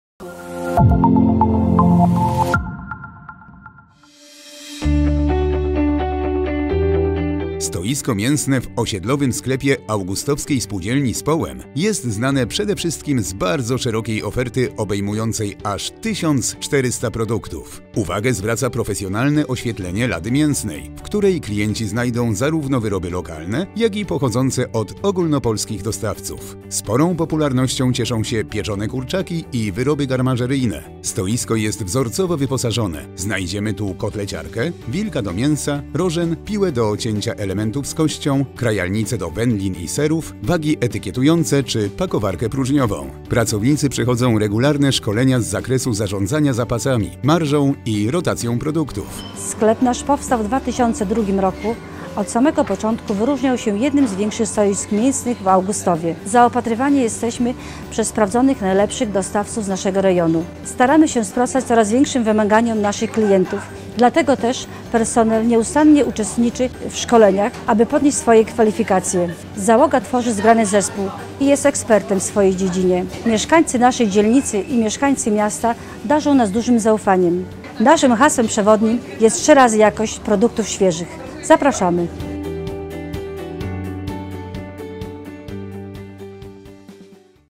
Konkurs na Najlepsze Stoisko Mięsno-Wędliniarskie 2020 - Społem Augustów
Stoiska z ladą mięsną o długości do 10 metrów.
Laureat Konkursu na Najlepsze Stoisko Mięsno-Wędliniarskie 2020
Społem
ul. Nowomiejska 83, 16-300 Augustów